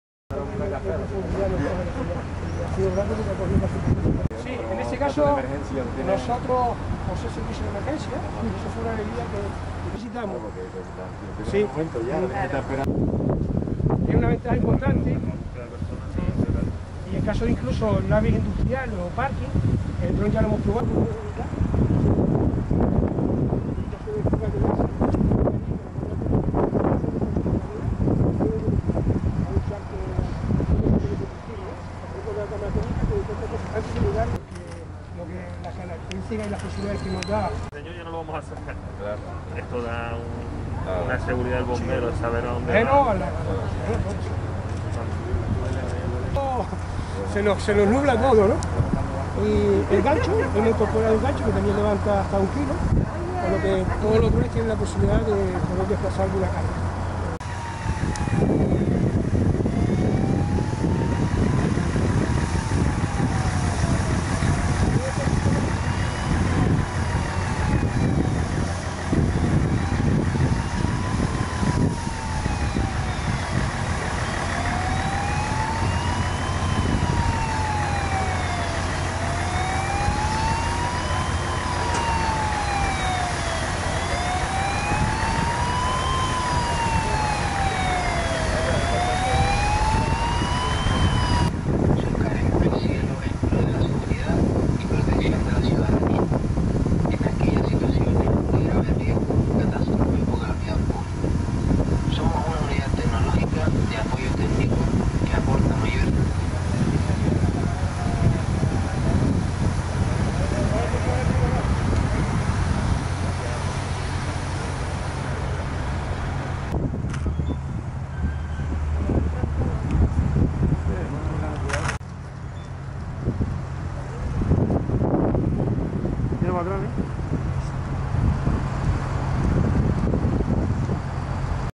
Presentación de los drones de los Bomberos de Las Palmas GC
El alcalde de Las Palmas de Gran Canaria, Augusto Hidalgo, y el concejal de Seguridad y Emergencias, Josué Íñiguez, junto a la jefa del Servicio de Extinción de Incendios y Salvamento (SEIS), Rosa Rodríguez, asistieron este lunes, 8 de marzo, a la presentación de los tres nuevos drones del Cuerpo de bomberos, coincidiendo con la festividad de San Juan de Dios, patrón de los Cuerpos de Bomberos. La presentación de los nuevos aparatos, que permiten seguir modernizando los recursos del cuerpo, tuvo lugar en el frente marítimo de Santa Catalina.